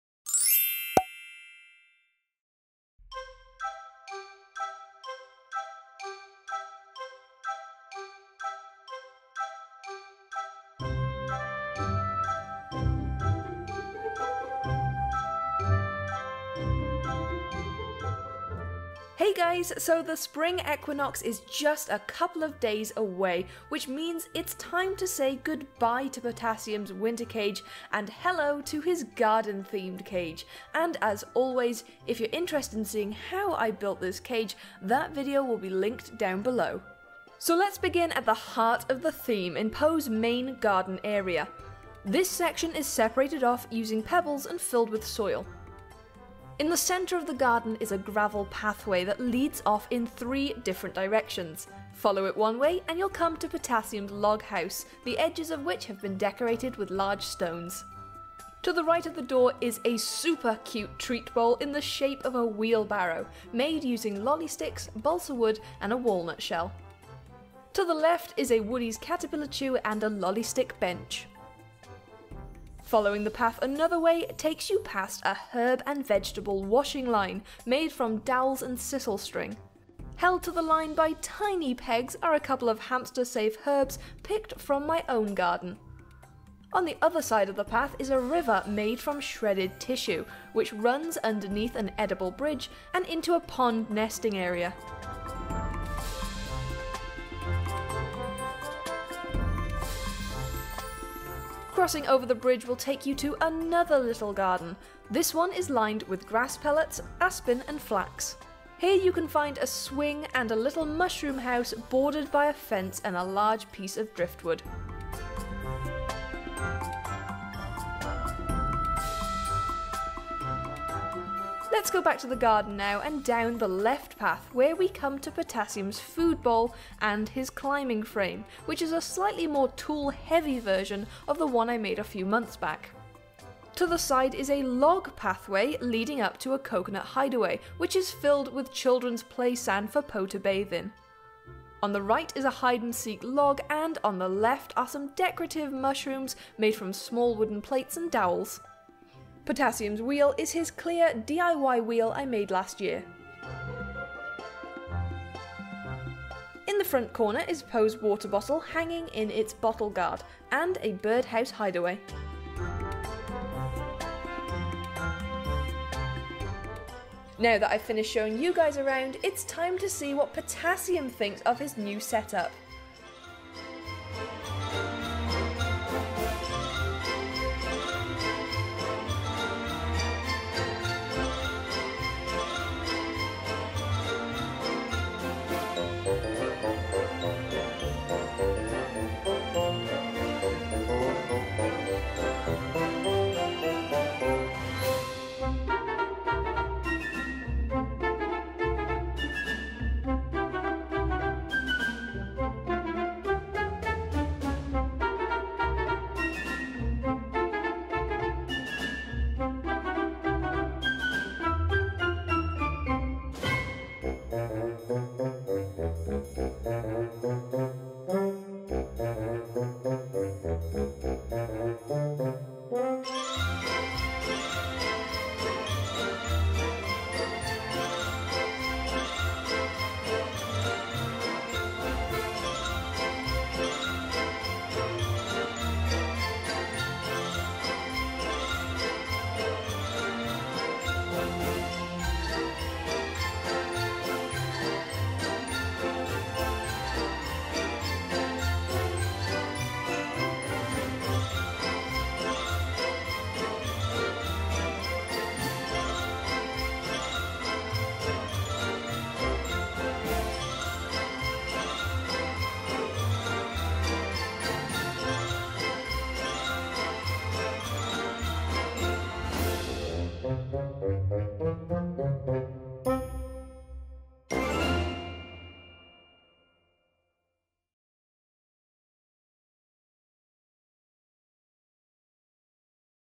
GARDEN Themed Hamster Cage + Cute Dwarf Hamster Exploring!
Welcome to Potassium the dwarf hamster's garden themed cage!

EQUIPMENT
+ Nikon D7000 (Main Camera)
+ Canon G7X II (Vlog Camera)
+ Sigma 35mm F1.8 (Lens)
+ Rode VideoMic (Micrphone)